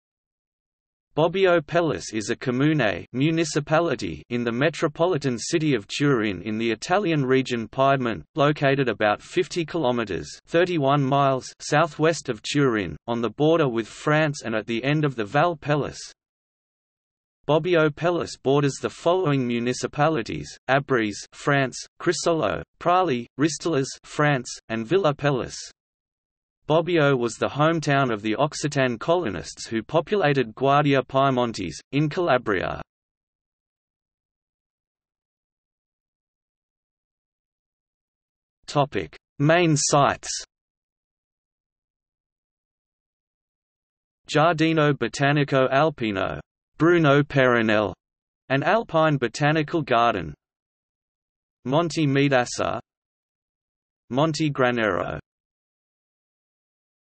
Bobbio Pellice | Wikipedia audio article
00:00:34 Main sights

- Socrates

SUMMARY
Bobbio Pellice is a comune (municipality) in the Metropolitan City of Turin in the Italian region Piedmont, located about 50 kilometres (31 mi) southwest of Turin, on the border with France and at the end of the Val Pellice
Bobbio Pellice borders the following municipalities: Abriès (France), Crissolo, Prali, Ristolas (France), and Villar Pellice. Bobbio was the hometown of the Occitan colonists who populated Guardia Piemontese, in Calabria.